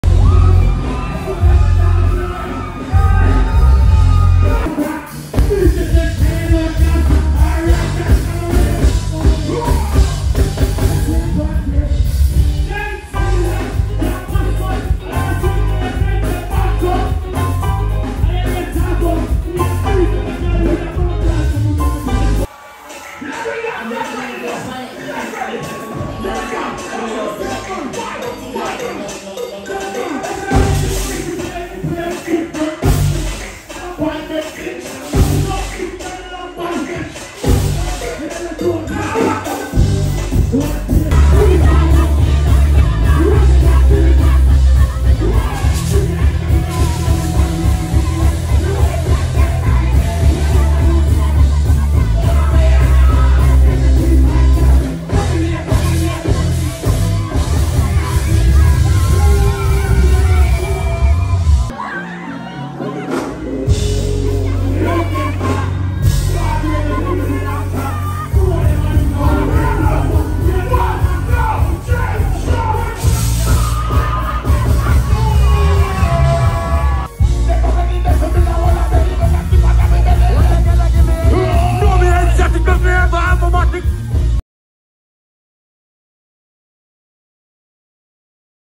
Skeng performing in Barbados at the Hennessy Artistry Show 2022
Skeng performing to a massive crowd in Barbados at the Hennessy Artistry Show 2022.